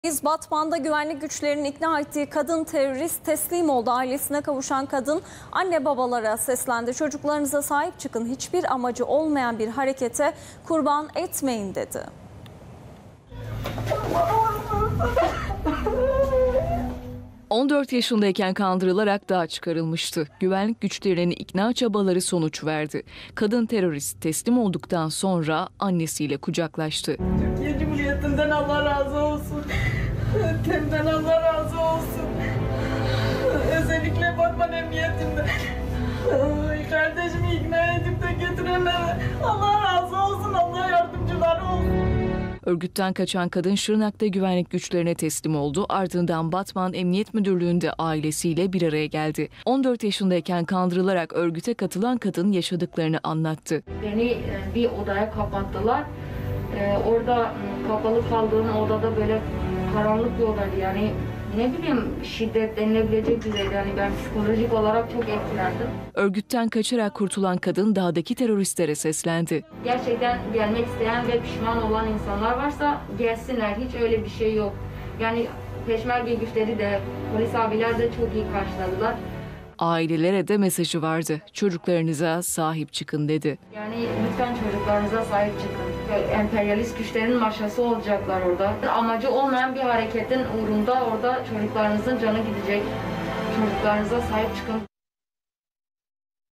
14 yaşında PKK'ya katılan kadın terörist yıllar sonra ailesine kavuştu
Batman'da güvenlik güçlerinin ikna çabaları sonucu terör örgütünden kaçarak teslim olan PKK'lı kadın, ailesiyle buluşturuldu.